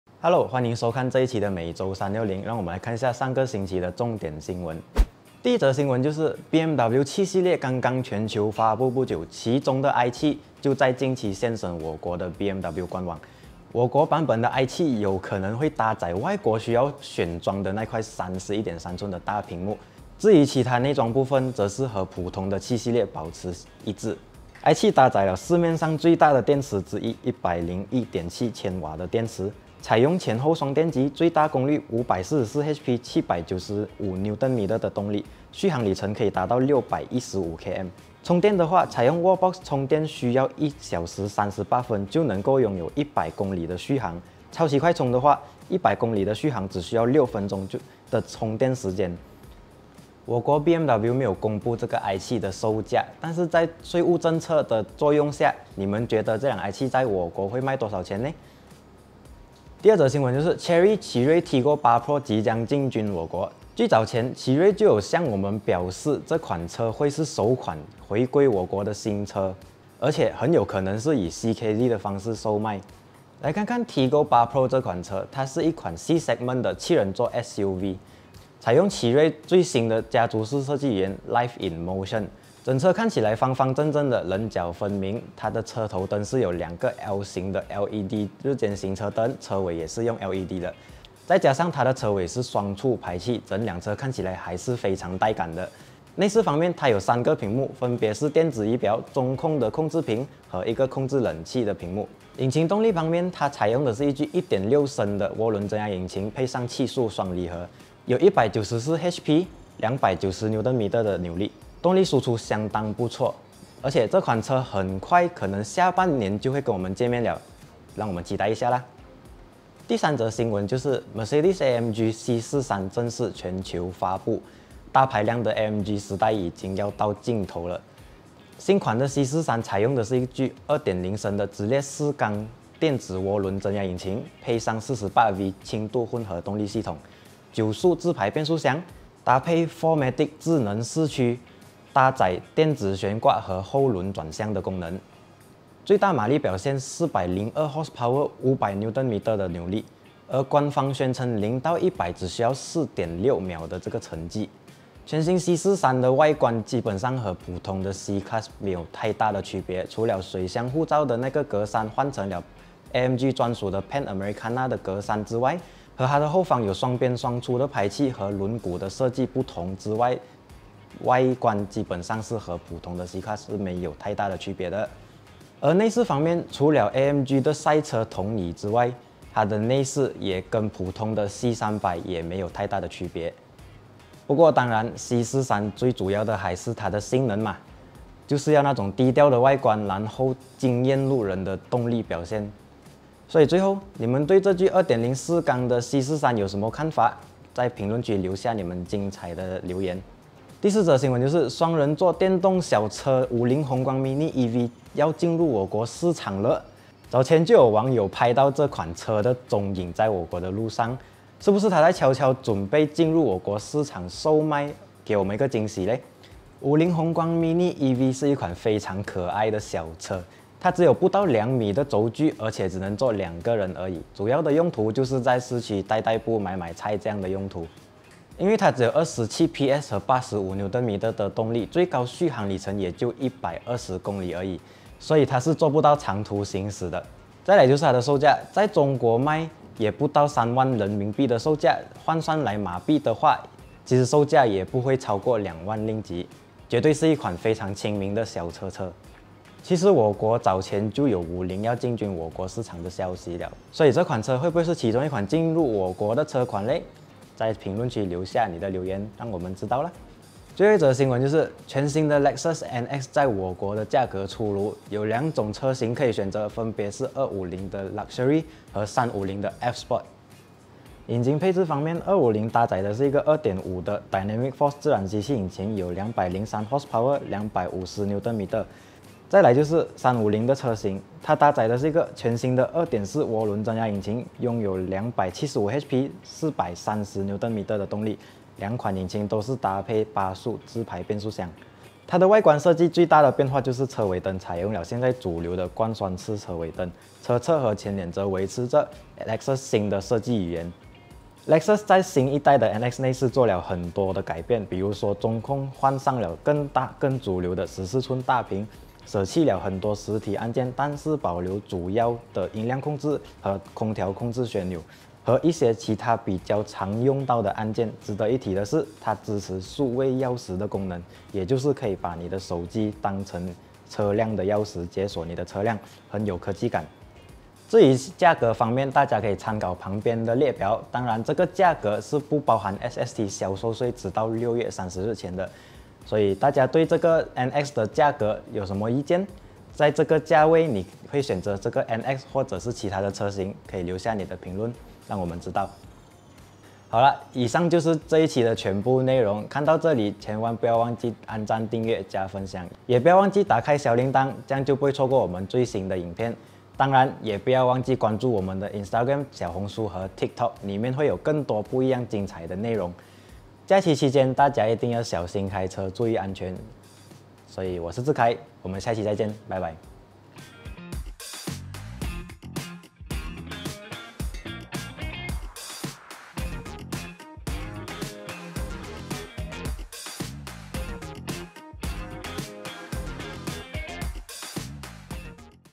2022 Lexus NX350 Turbo 确定来马！275 Hp的日系运动SUV你期待吗？（每周360）｜automachi.com 马来西亚试车频道
1. BMW i7 现身我国官网
2. 五菱宏光 Mini EV 现身我国
3. Chery Tiggo 8 Pro 即将进军我国
4. AMG C43 全球发布
5. Lexus NX 我国价格出炉